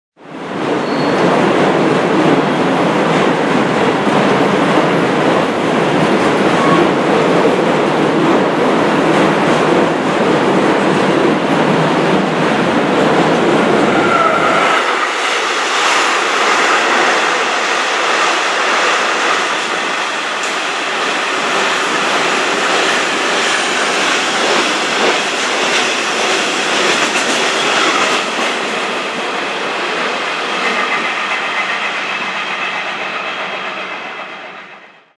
MRCE 189 451-8 (Siemens ES 64 F4-451) mit einem leerem Niag Kohlezug
Aufgenommen am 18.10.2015 in Saarbrücken-Jägersfreude